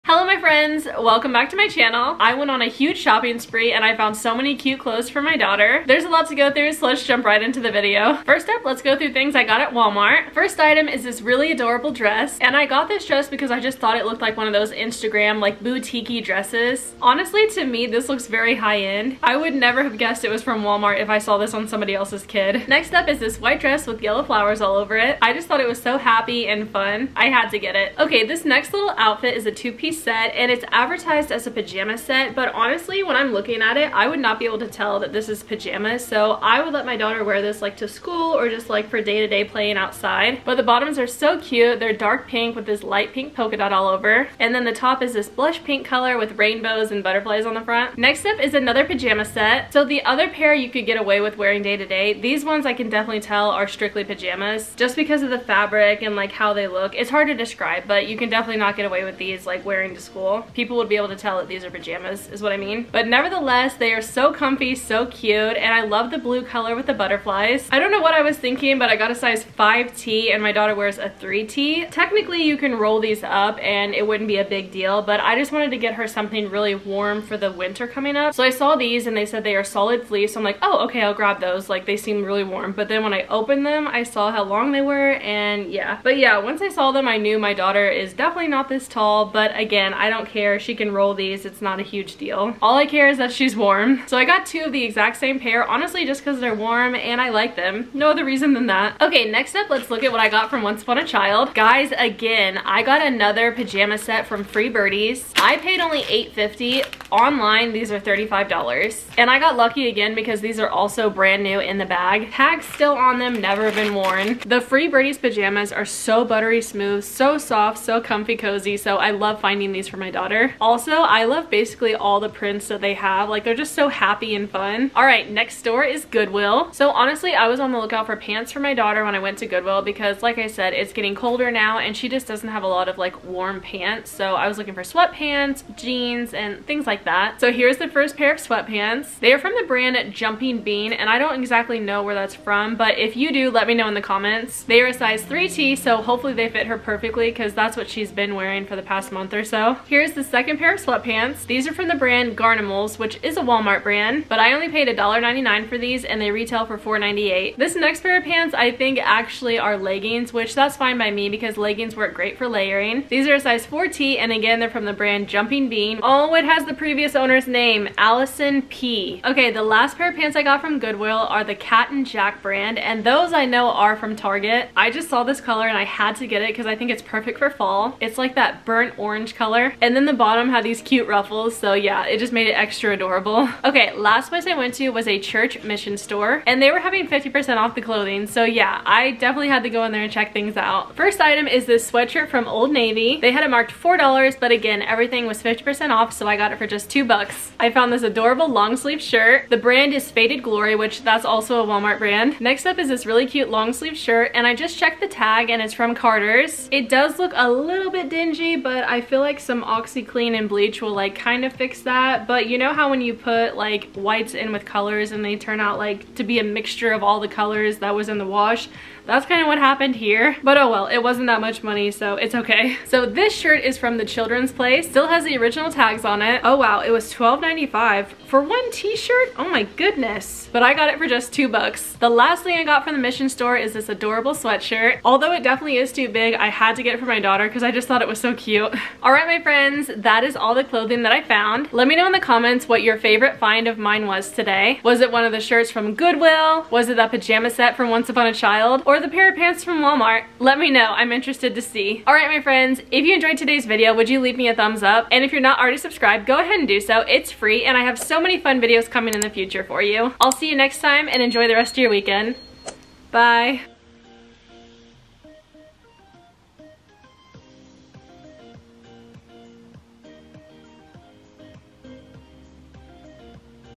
MASSIVE KIDS CLOTHING HAUL/GOODWILL/WALMART/ONCE UPON A CHILD
Hello Friends!

I went shopping..again lol This time in search for clothes for my daughter! I scored big time and got great deals along the way.  You don't want to miss this massive haul!